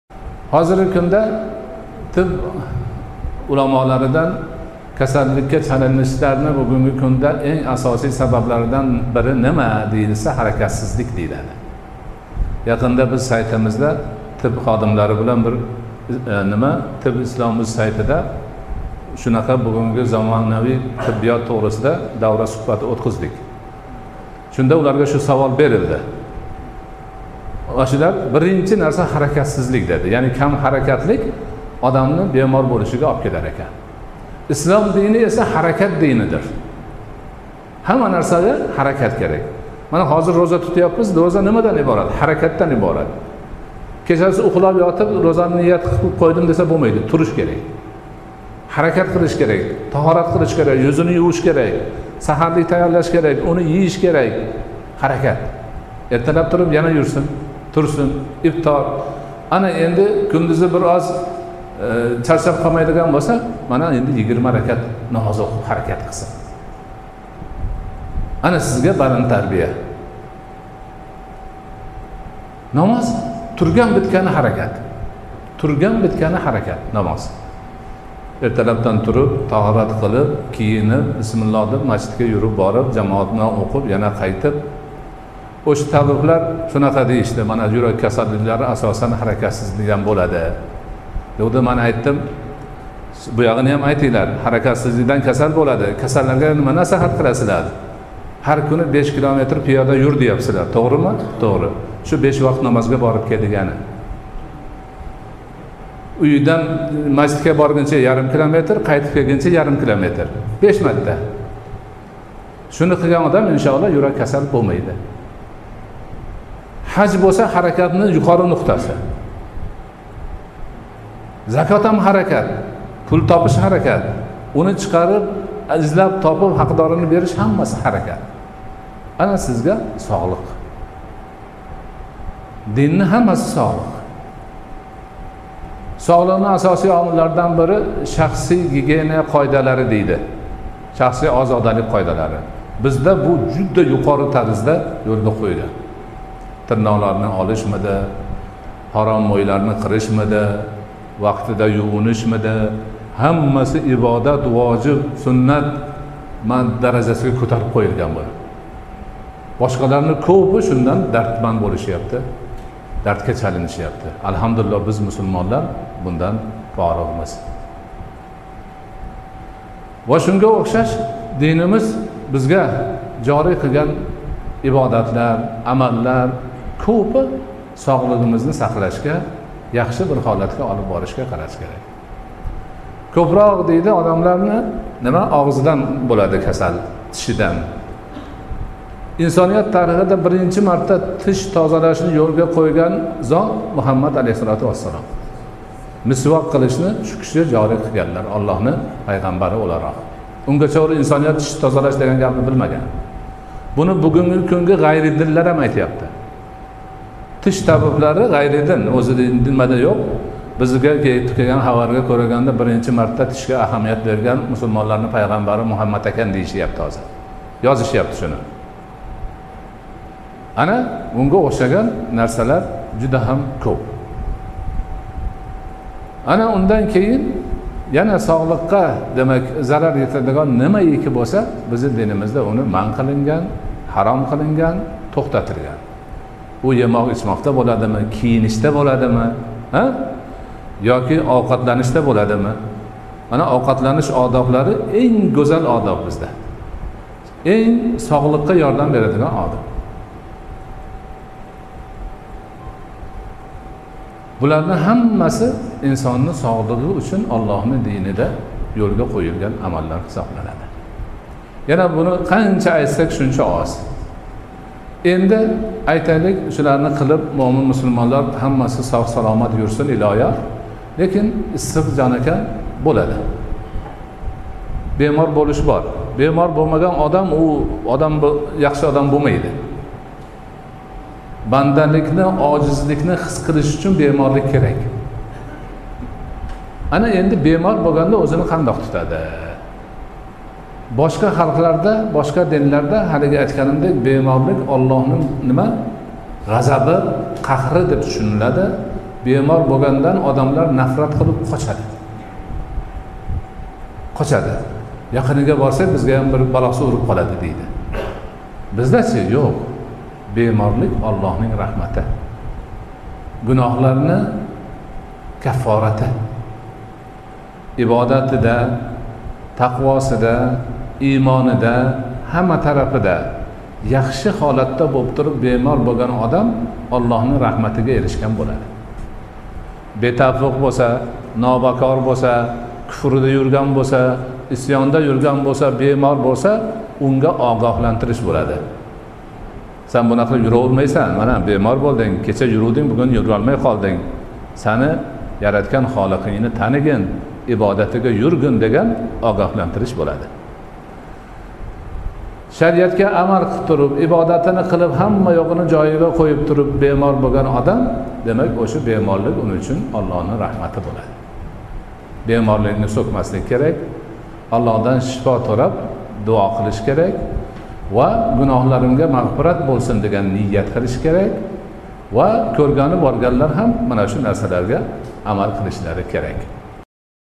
КАСАЛЛИКЛАРГА ЧАЛИНИШНИНГ АСОСИЙ САБАБЛАРИ! - ШАЙХ МУҲАММАД СОДИҚ МУҲАММАД ЮСУФ роҳимаҳуллоҳ
Манба: Islom.uz